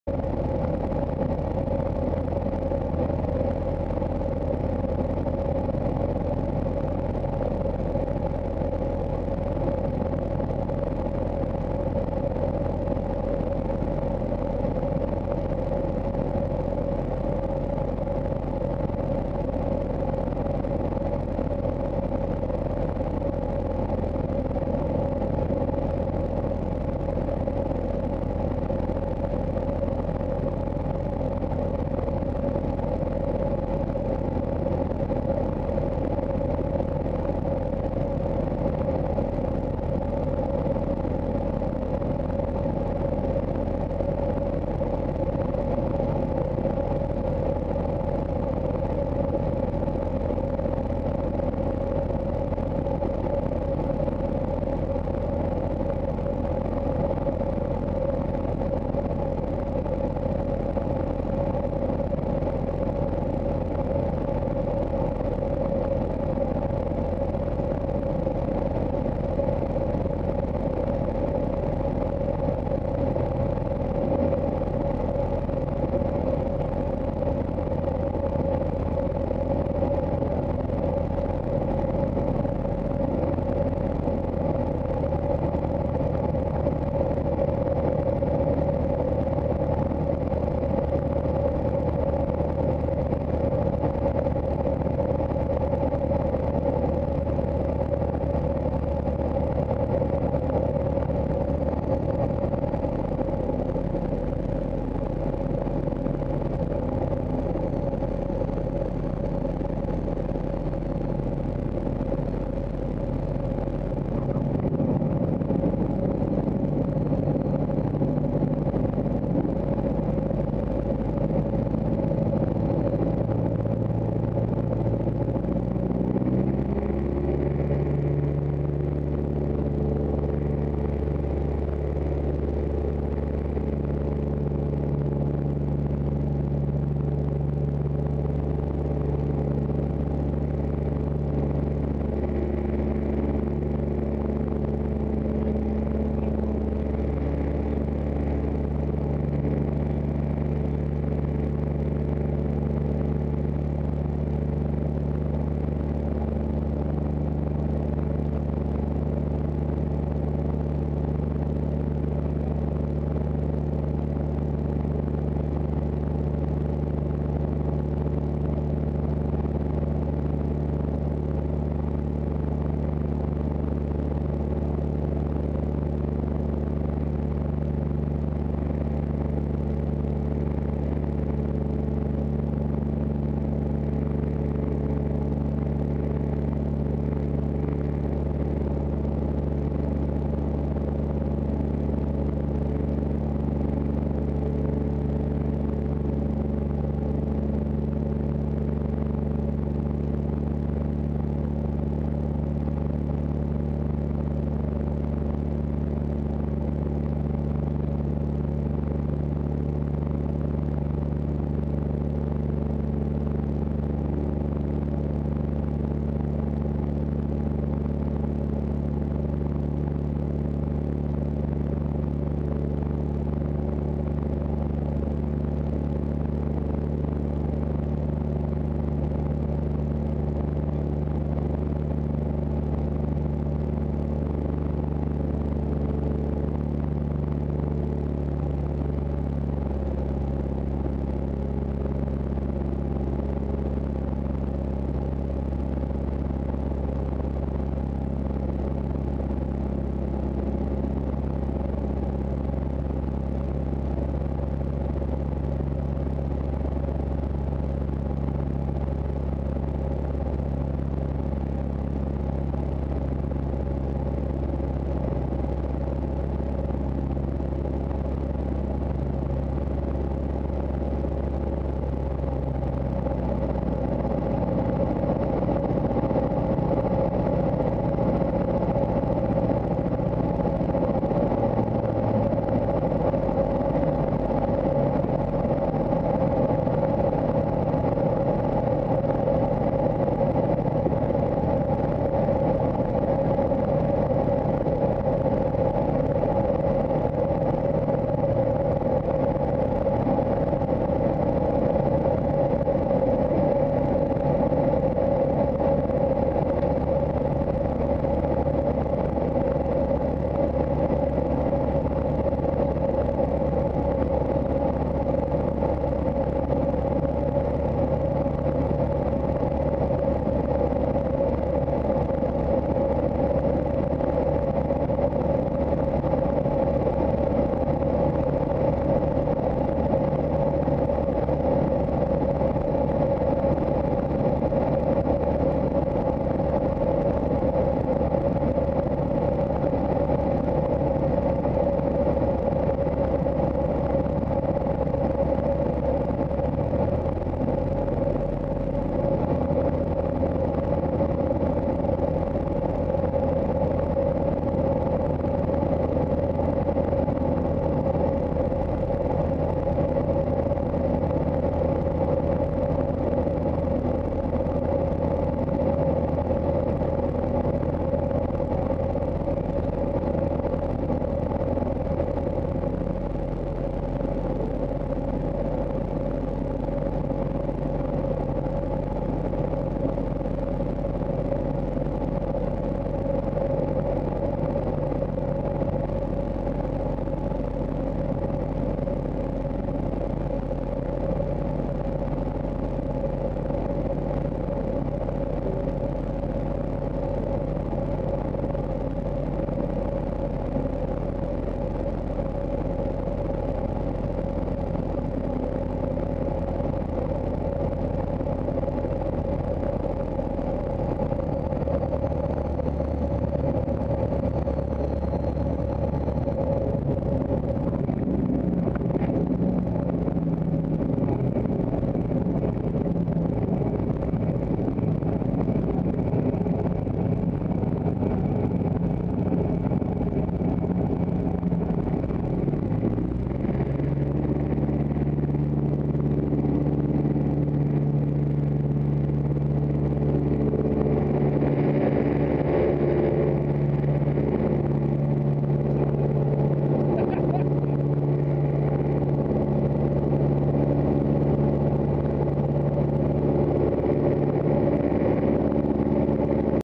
RV-6 Flight 8-4-13
45 Entry To Down Wind, Base To Final, Landing & Go Around Marana Northwest Regional Runway 12